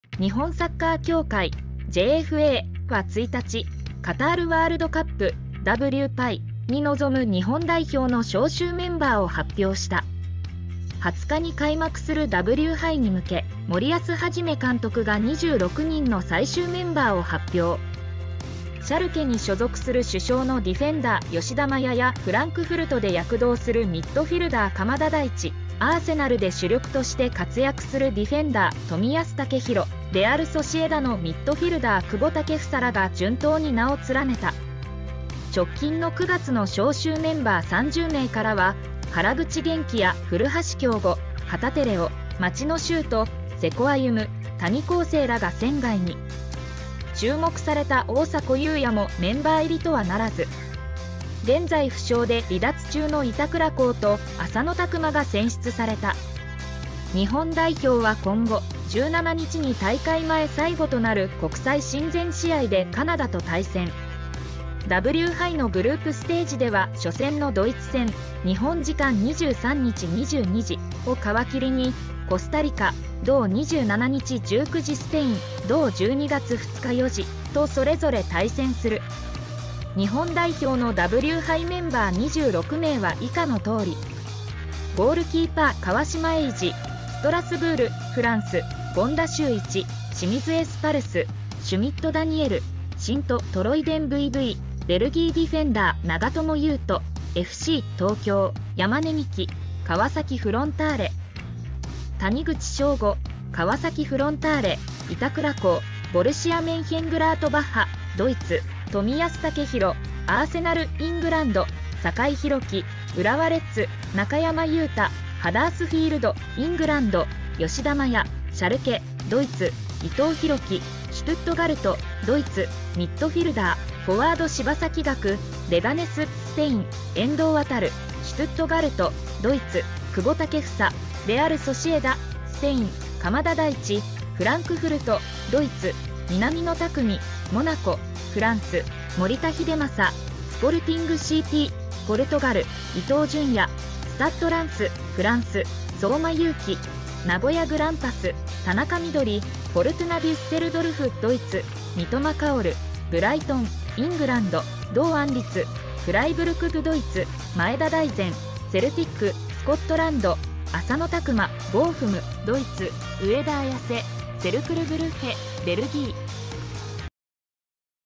「ニュース」サッカー日本代表、カタールW杯メンバー26名が発表！南野拓実・久保建英らが順当に選出
日本サッカー協会（JFA）は1日、カタール・ワールドカップ（W杯）に臨む日本代表の招集メンバーを発表した。20日に開幕するW杯に向け、森保一監督が26人の最終メンバーを発表。シャルケに所属する主将のDF吉田麻也やフランクフルトで躍動するMF鎌田大地、アーセナルで主力として活躍するDF冨安健洋、レアル・ソシエダのMF久保建英らが順当に名を連ねた。直近の9月の招集メンバー30名からは、原口元気や古橋亨梧、旗手怜央、町野修斗、瀬古歩夢、谷晃生らが選外に。注目された大迫勇也もメンバー入りとはならず。現在負傷で離脱中の板倉滉と浅野拓磨が選出された。日本代表は今後、17日に大会前最後となる国際親善試合でカナダと対戦。W杯のグループステージでは初戦のドイツ戦（日本時間23日22時）を皮切りに、コスタリカ（同27日19時）、スペイン（同12月2日4時）とそれぞれ対戦する。日本代表のW杯メンバー26名は以下のとおり。▽GK・川島永嗣（ストラスブール/フランス）・権田修一（清水エスパルス）・シュミット・ダニエル（シント=トロイデンVV/ベルギー）▽DF・長友佑都（FC東京）・山根視来（川崎フロンターレ）・谷口彰悟（川崎フロンターレ）・板倉滉（ボルシア・メンヒェングラートバッハ/ドイツ）・冨安健洋（アーセナル/イングランド）・酒井宏樹（浦和レッズ）・中山雄太（ハダースフィールド/イングランド）・吉田麻也（シャルケ/ドイツ）・伊藤洋輝（シュトゥットガルト/ドイツ）▽MF/FW・柴崎岳（レガネス/スペイン）・遠藤航（シュトゥットガルト/ドイツ）・久保建英（レアル・ソシエダ/スペイン）・鎌田大地（フランクフルト/ドイツ）・南野拓実（モナコ/フランス）・守田英正（スポルティングCP/ポルトガル）・伊東純也（スタッド・ランス/フランス）・相馬勇紀（名古屋グランパス）・田中碧（フォルトゥナ・デュッセルドルフ/ドイツ）・三笘薫（ブライトン/イングランド）・堂安律（フライブルク/ドイツ）・前田大然（セルティック/スコットランド）・浅野拓磨（ボーフム/ドイツ）・上田綺世（セルクル・ブルッヘ/ベルギー）